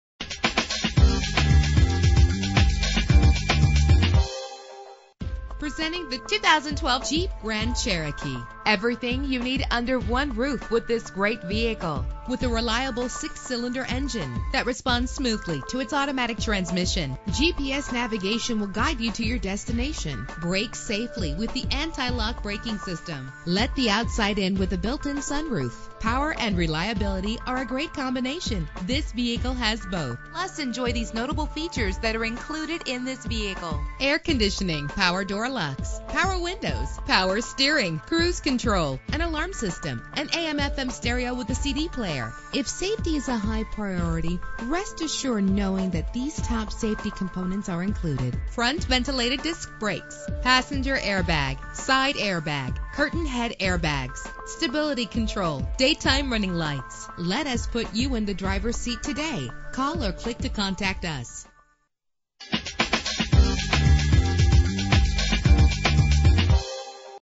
2012 Jeep Grand Cherokee - Ocala FL
Year: 2012
Make: Jeep
Model: Grand Cherokee
Trim: Overland
Engine: 3.6 liter 6 cylinder 24 valve
Transmission: Automatic
Color: Black Forest Green Pearlcoat
Mileage: 0
Address:
3440 South Pine Avenue